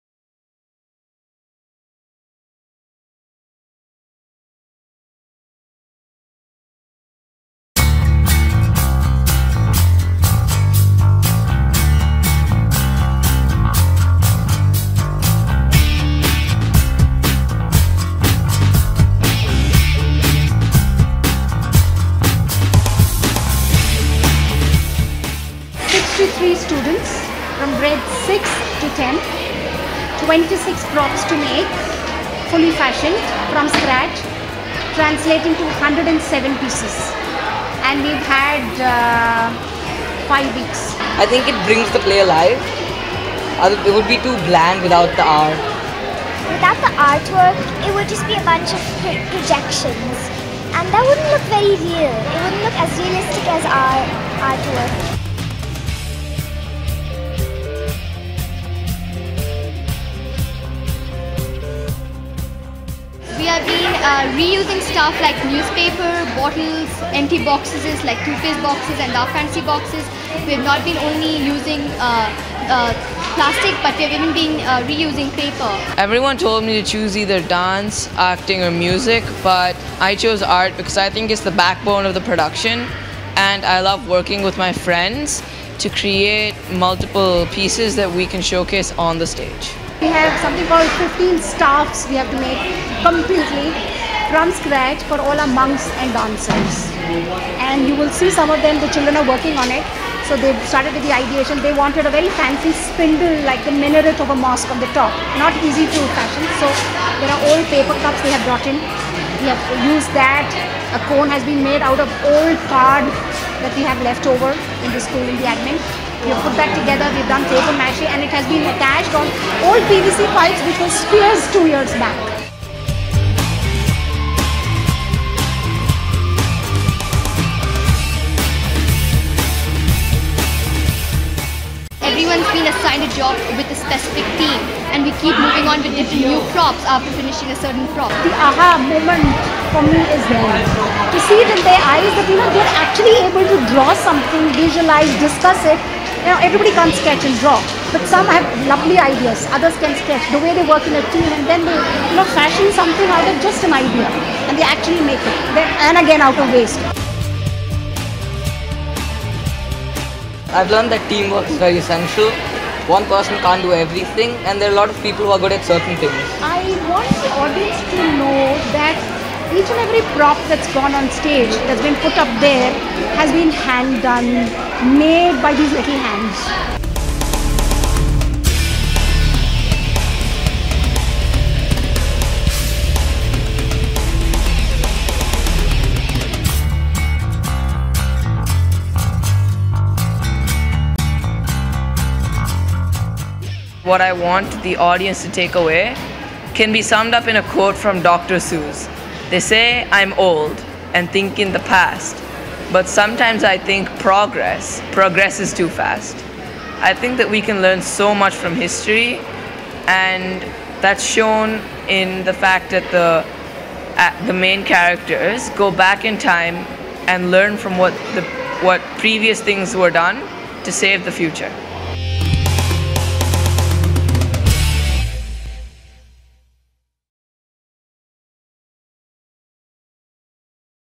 Beyond The Bubble : Art, everything you see is handmade- Inventure Academy
Furious practice has gripped our senior school as they perfect their lines, their moves, their music & their cues for the production.

The backdrops and artwork for the play have been painstakingly done by the students at Inventure. Even digital prints prototypes were drawn by hand.

Learning outside the class room and what it all entails, hear it from the students.